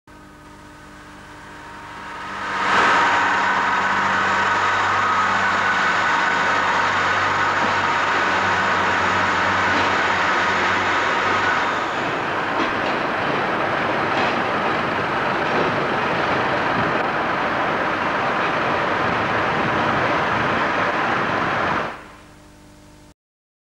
PROVINCIAL.Class 142 Departing from Manchester Victoria 19. 8. 91. Neil Hayton railway memories.
E.C.S move for class 142 departing from manchester victoria.